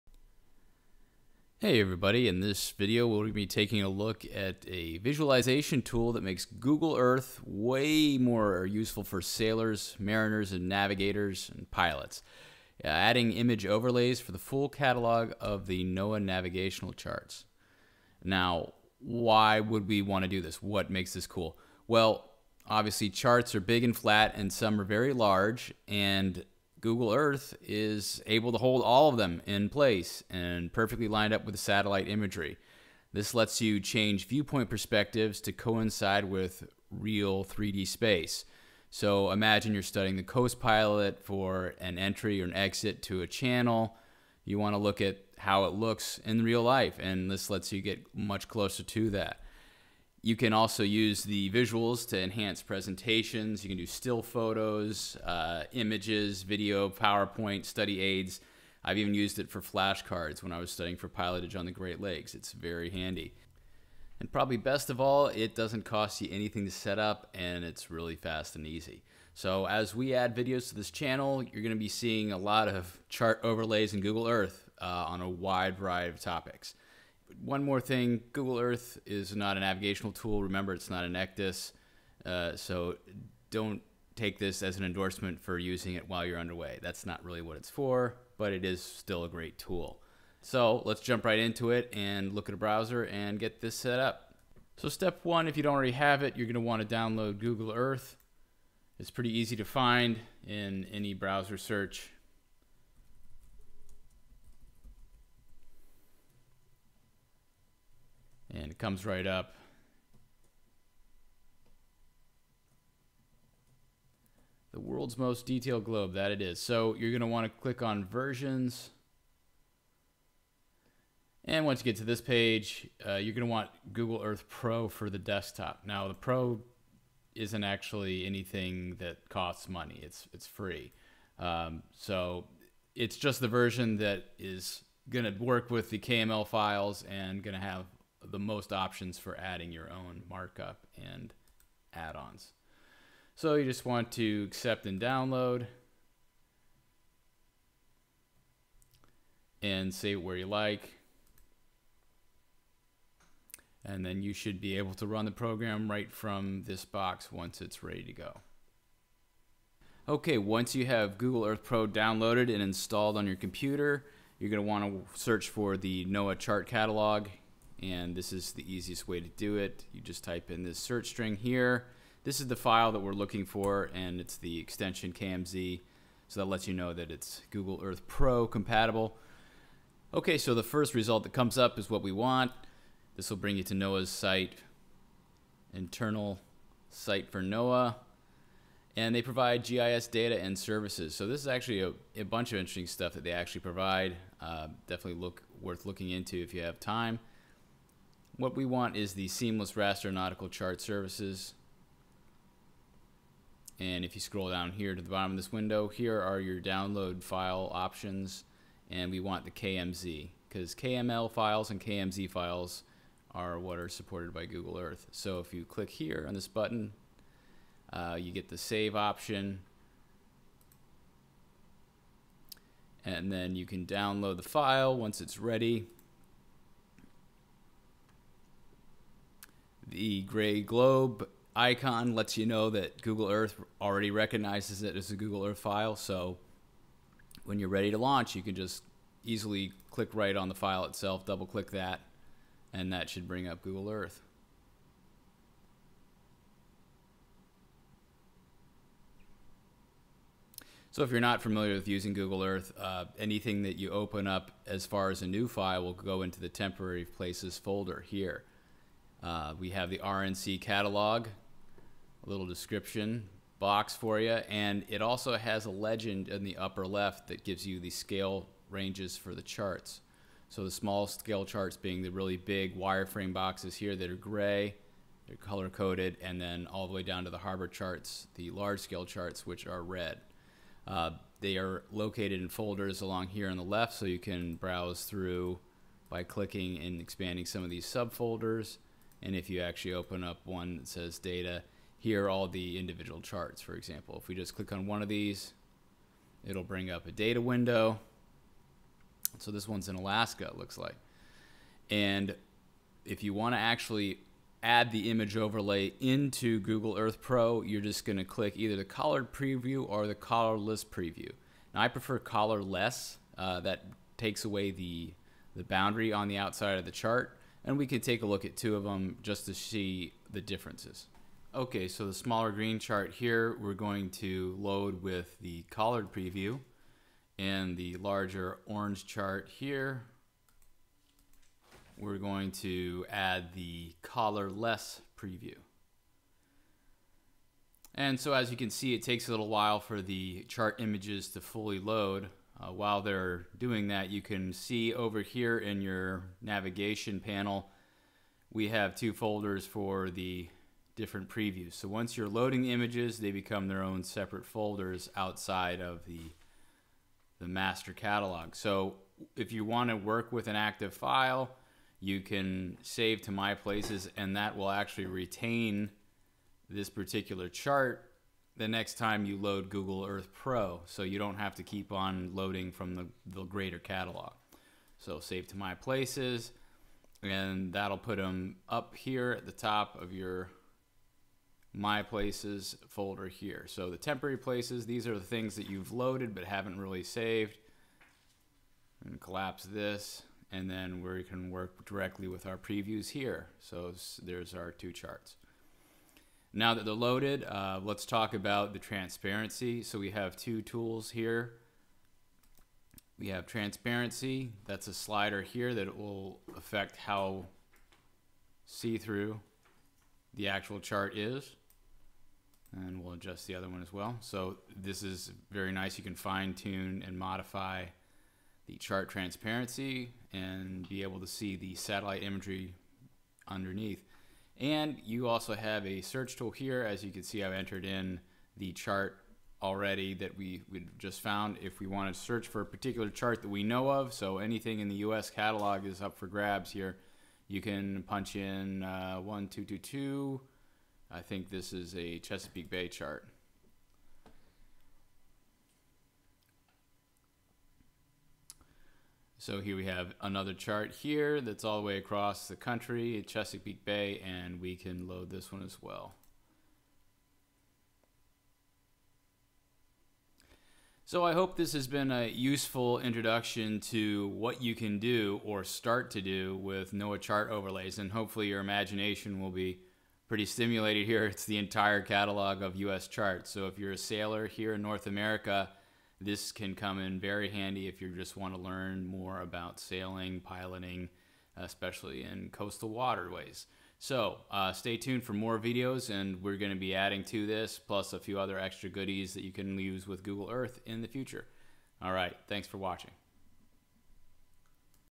Google Earth for Sailors: NOAA Chart Overlays
A quick tutorial for adding the full US Catalog of nautical charts into Google Earth.  Enhance your studies in US Navigation, Coastal Piloting, and Sailing.